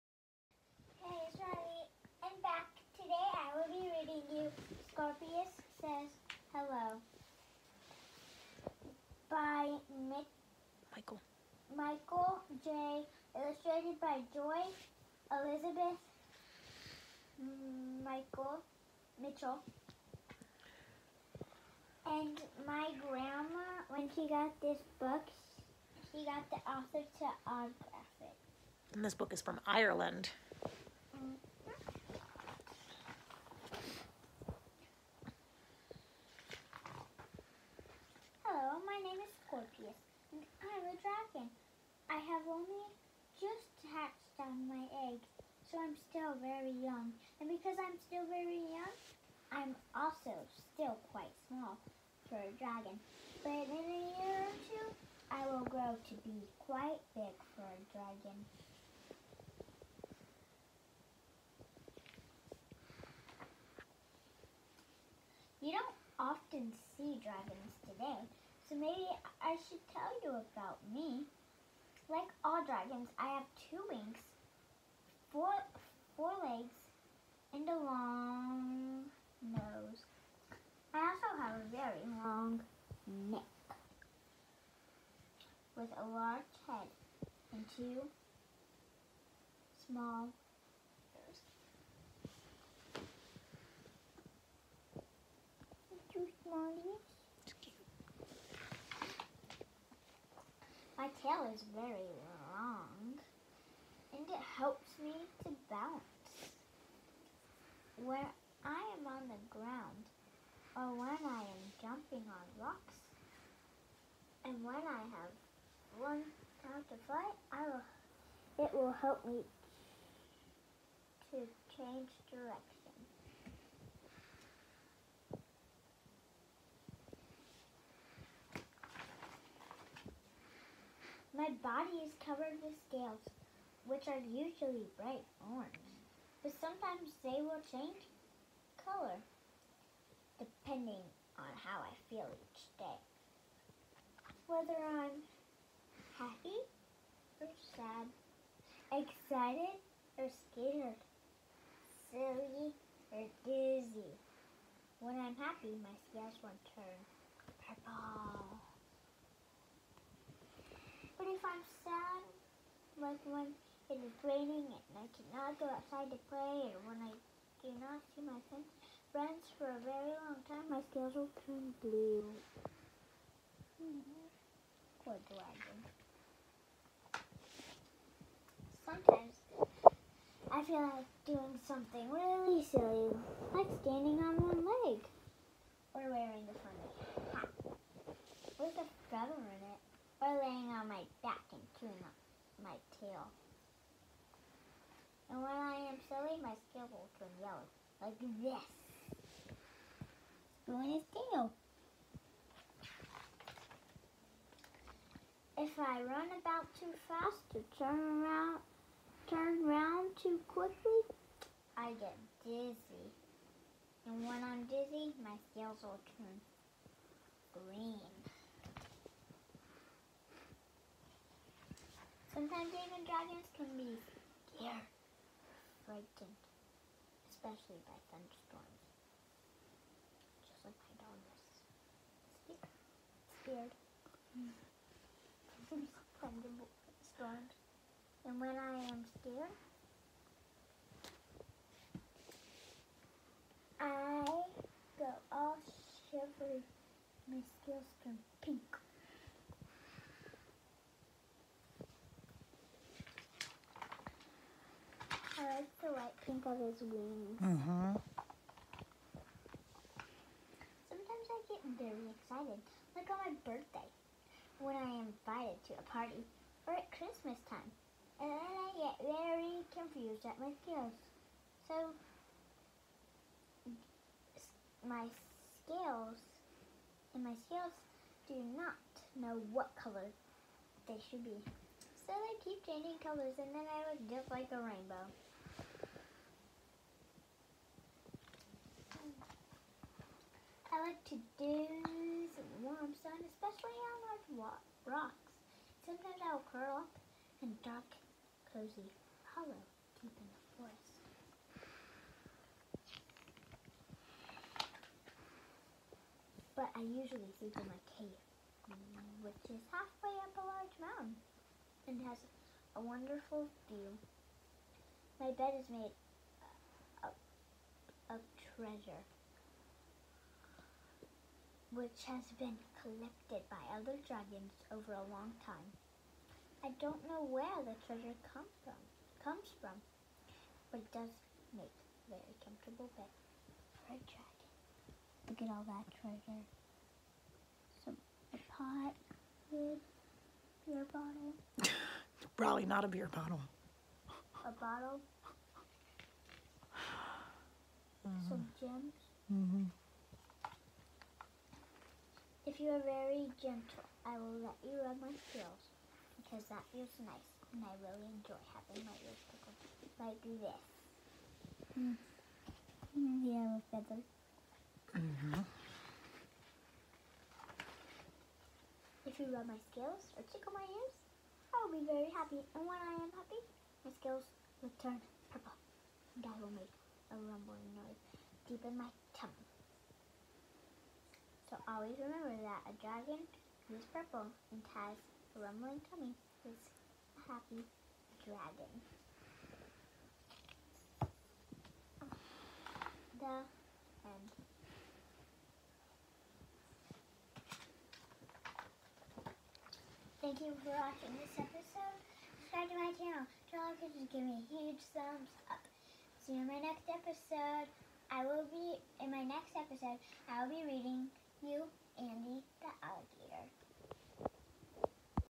Riley A’s Reading Room- Scorpius Says Hello
Silly book about a dragon, feelings and colors.